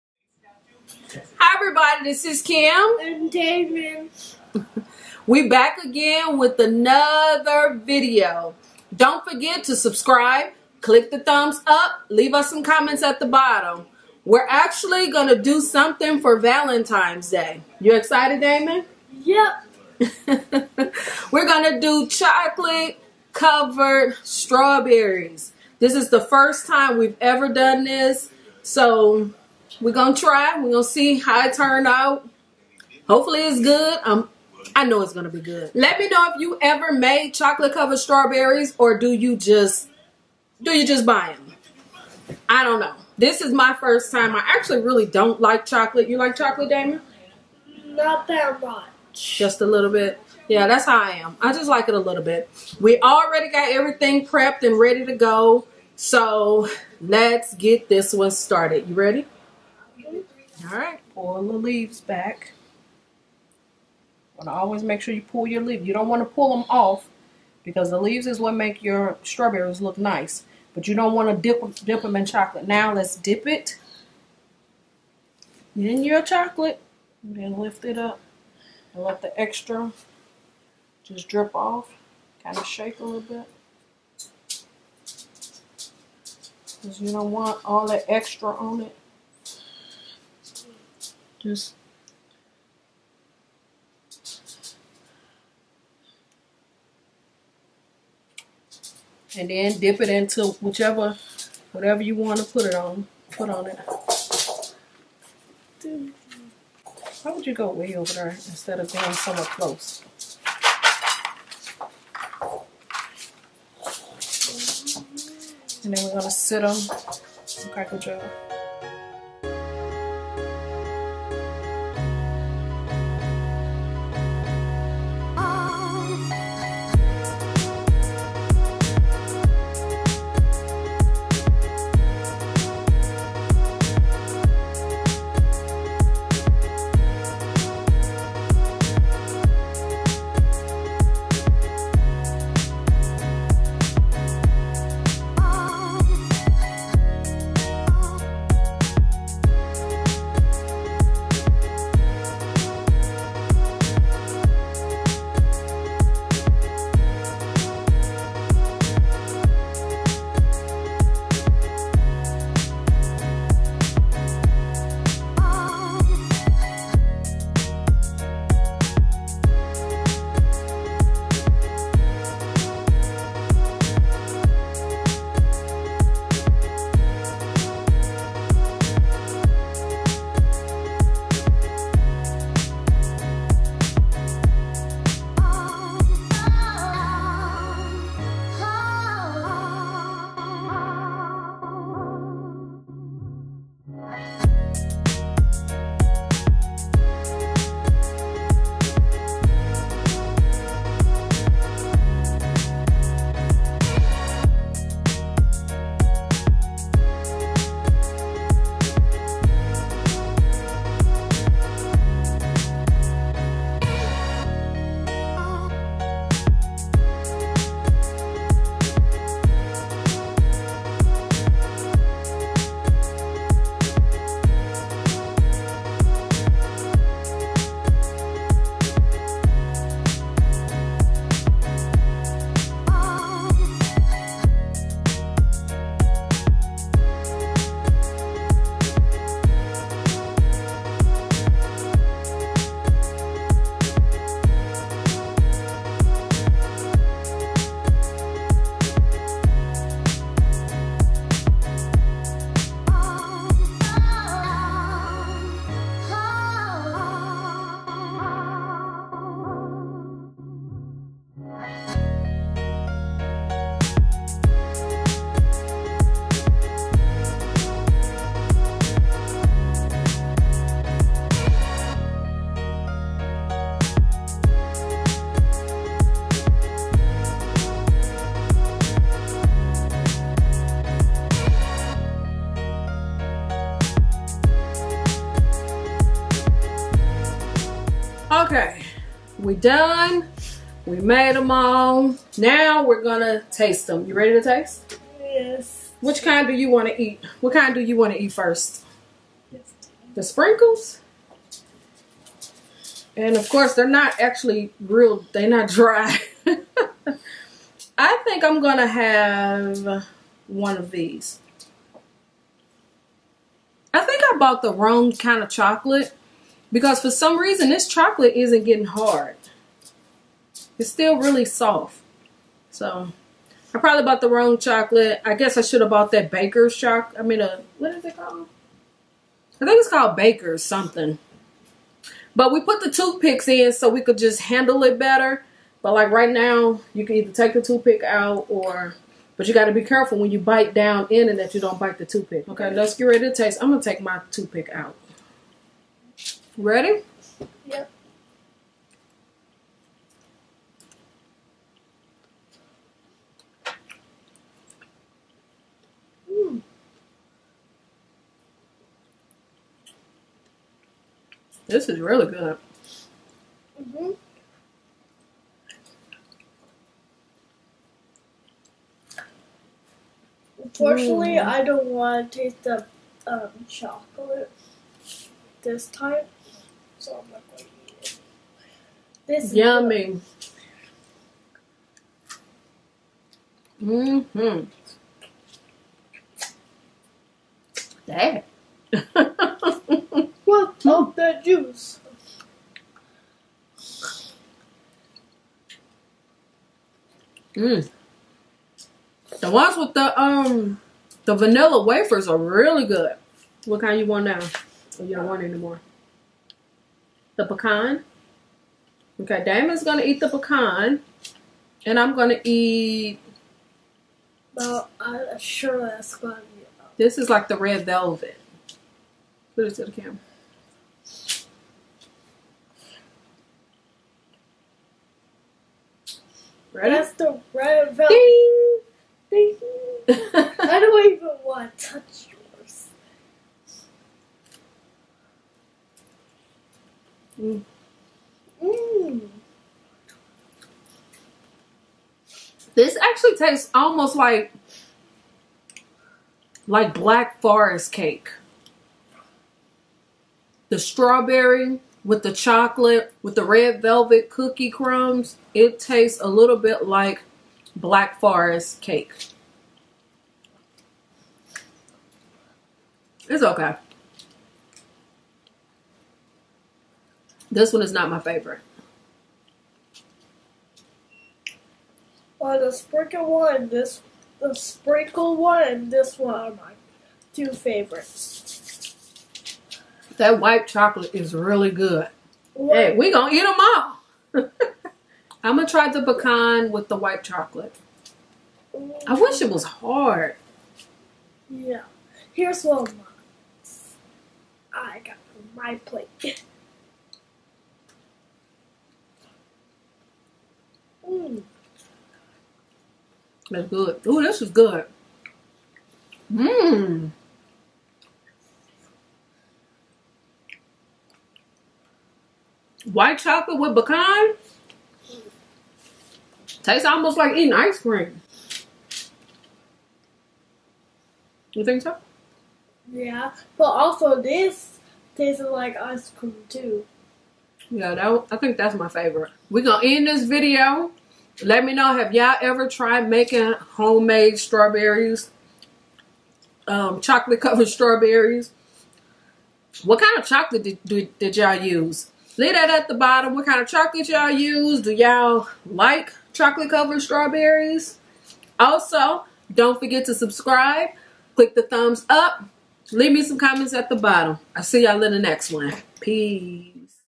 Chocolate Covered Strawberries | Easy recipe | Mom and Son Cooking
Damon and I making Chocolate Covered Strawberries. It was so easy to do and didn't take long at all.